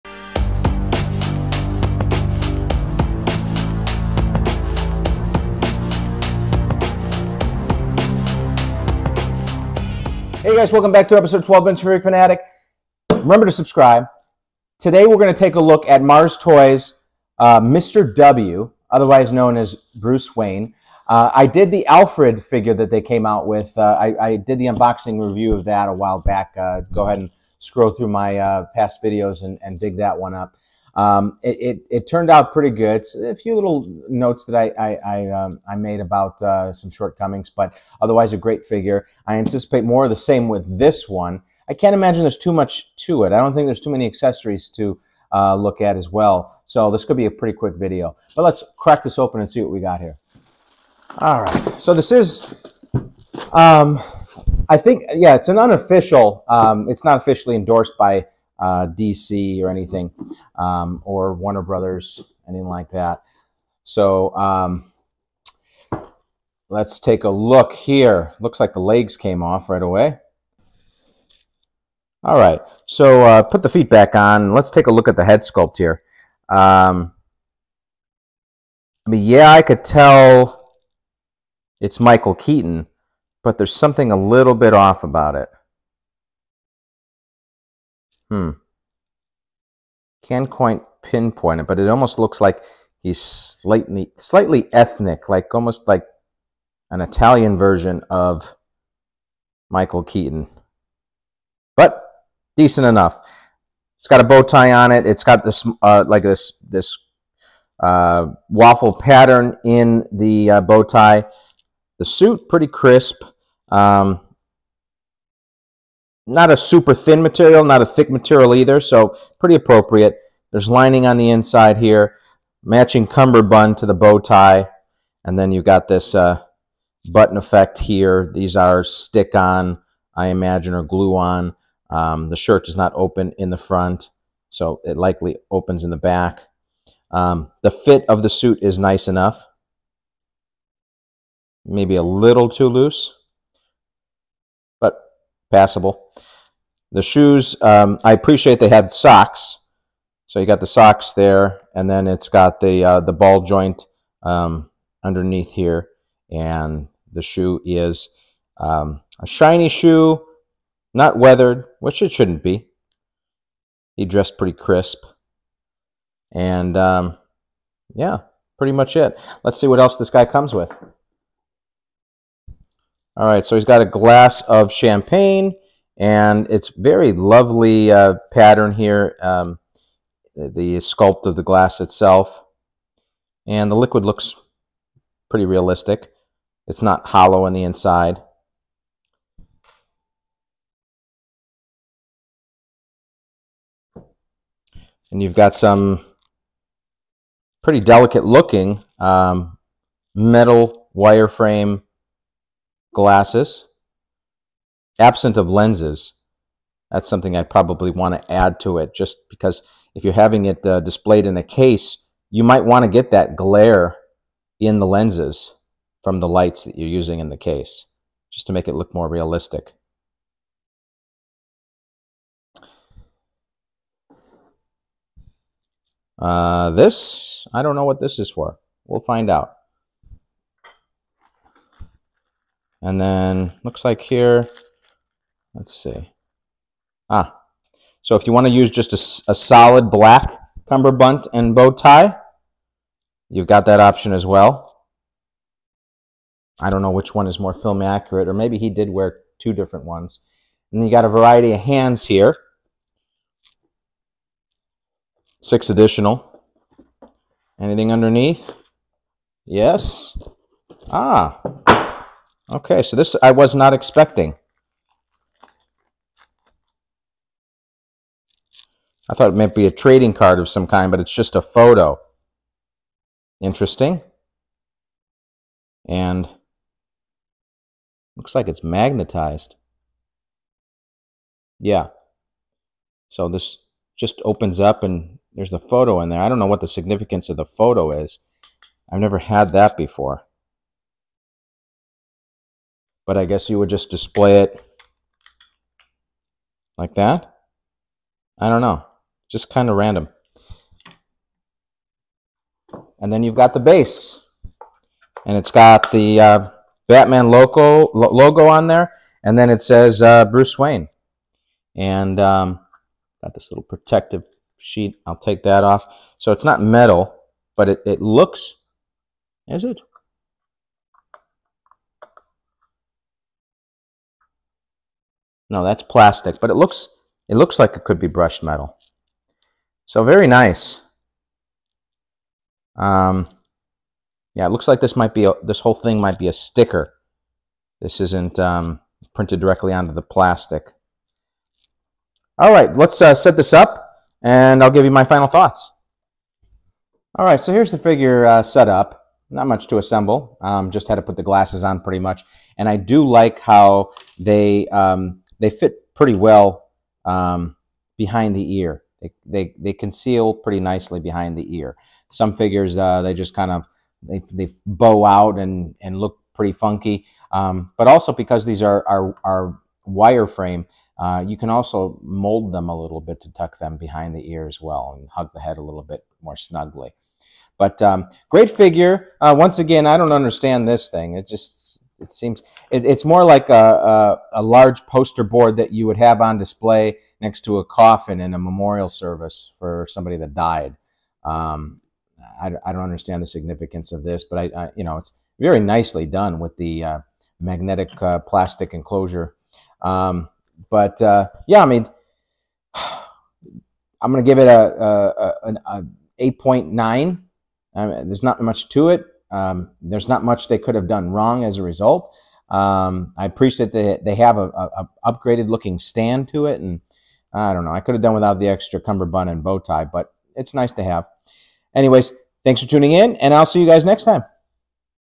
MARS TOYS "Mr. W" (Bruce Wayne) Unboxing & Review
A basic but decent figure to add toy your Batman figure collection